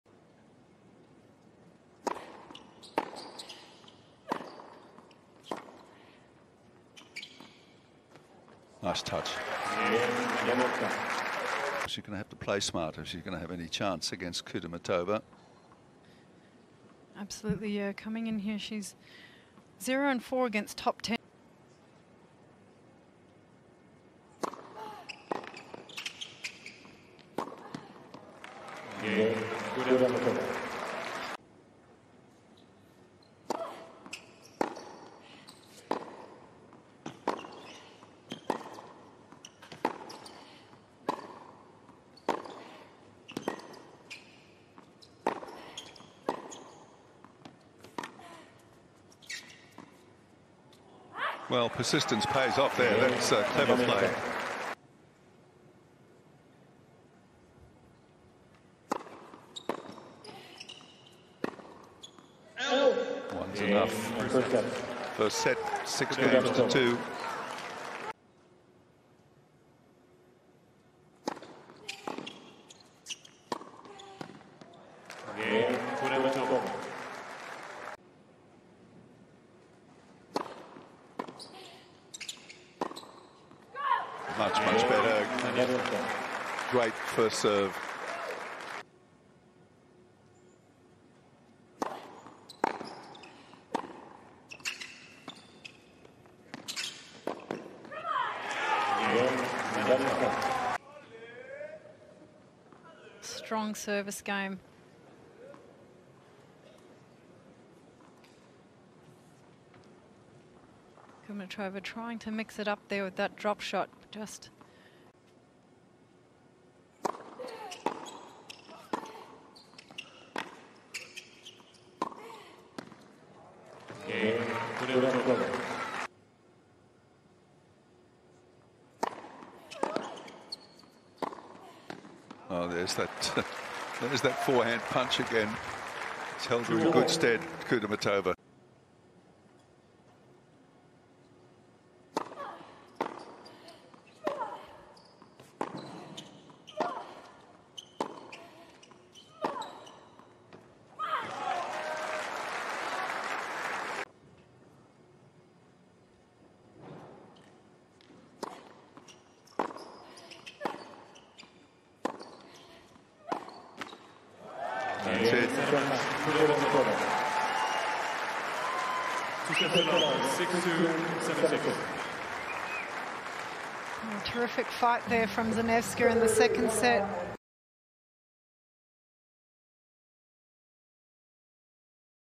Maryna Zanevska v Veronika Kudermetova Highlights | Australian Open 2023 First Round
Watch the highlights of Maryna Zanevska v Veronika Kudermetova in the first round of the Australian Open 2023.